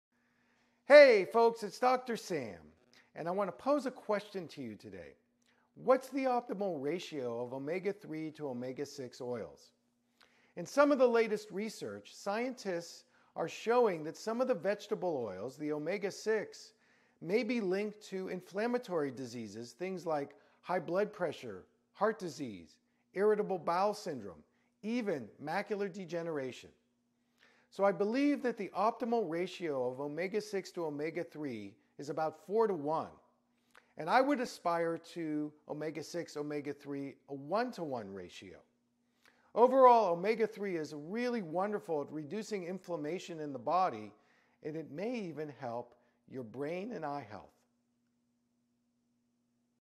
Omega-6 and Omega-3 Ratios
What's the optimal ratio of Omega-6 and Omega 3 oils to reduce inflammation in the body?